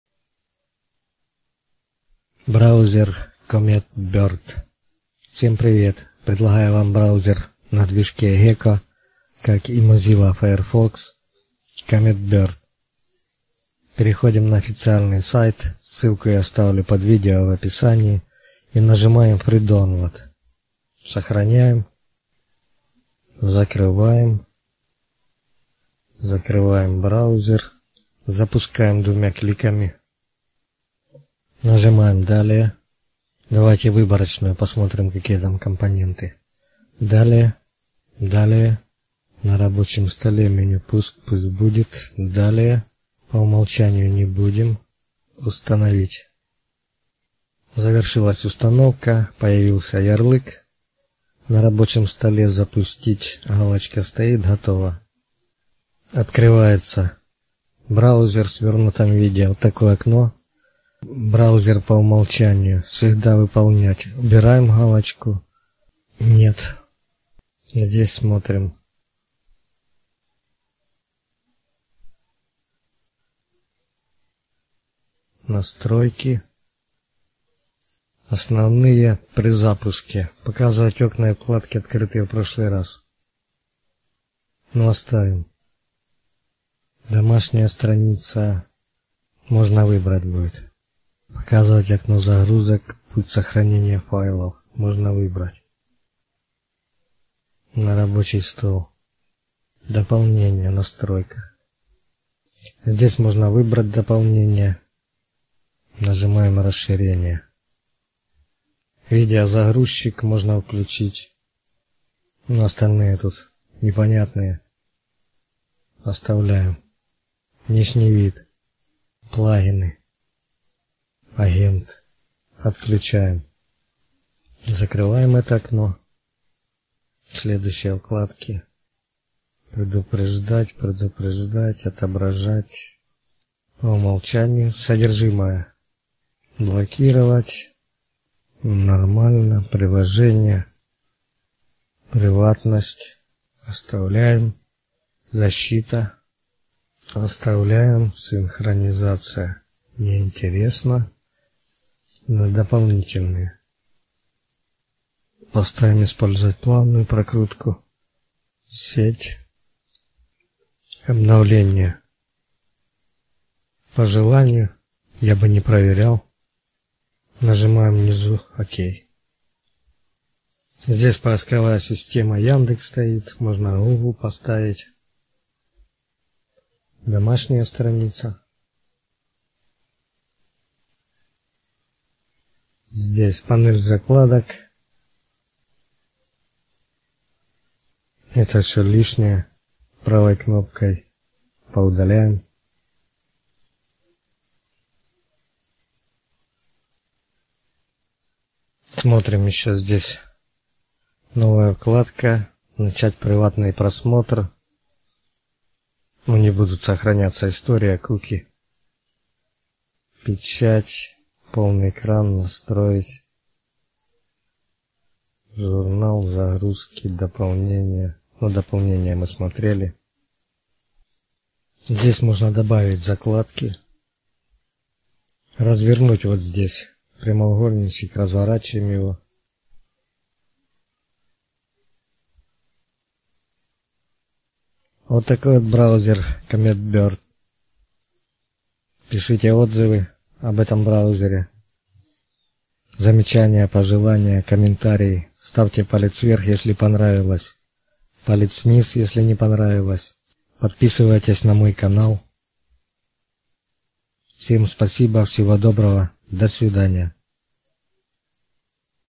Легкий браузер CometBird. Быстрый и безопасный браузер
Легкий браузер CometBird. Быстрый и безопасный браузер. Видео о том, как скачать и установить легкий, быстрый и безопасный браузер CometBird.
Официальный сайт, где можно скачать легкий, быстрый и безопасный браузер CometBird